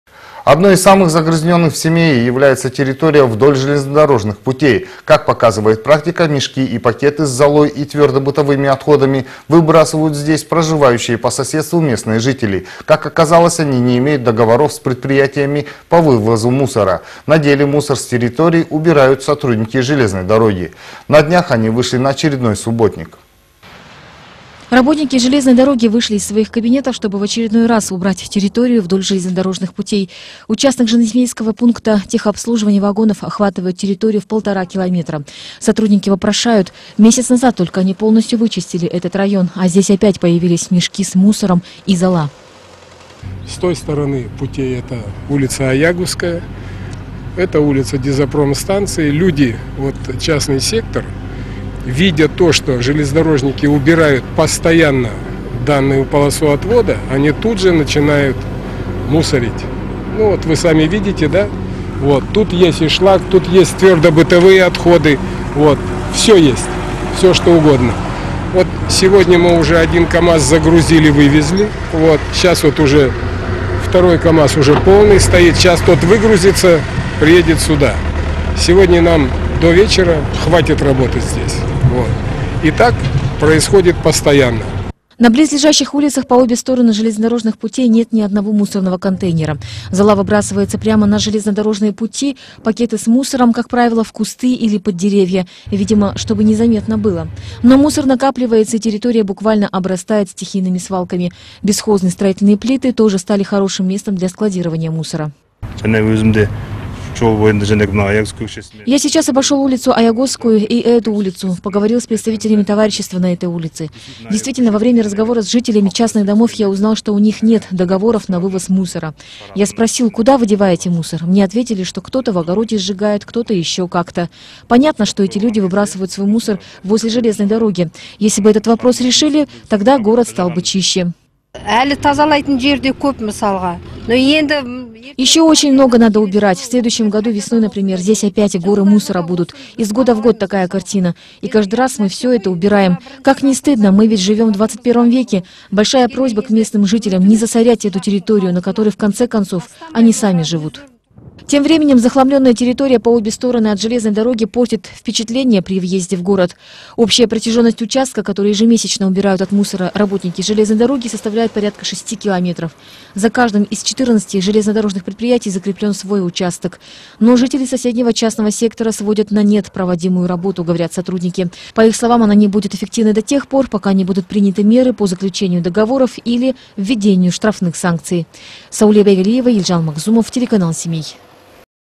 КТО НАВОДИТ ПОРЯДОК ВДОЛЬ ЖЕЛЕЗНОДОРПОЖНЫХ ПУТЕЙ?
Одной из самых загрязненных в Семее является территория вдоль железнодорожных путей. Как показывает практика, мешки и пакеты с золой и твердо-бытовыми отходами выбрасывают здесь проживающие по соседству местные жители. Как оказалось, они не имеют договоров с предприятиями по вывозу мусора. На деле мусор с территорий убирают сотрудники железной дороги. На днях они вышли на очередной субботник.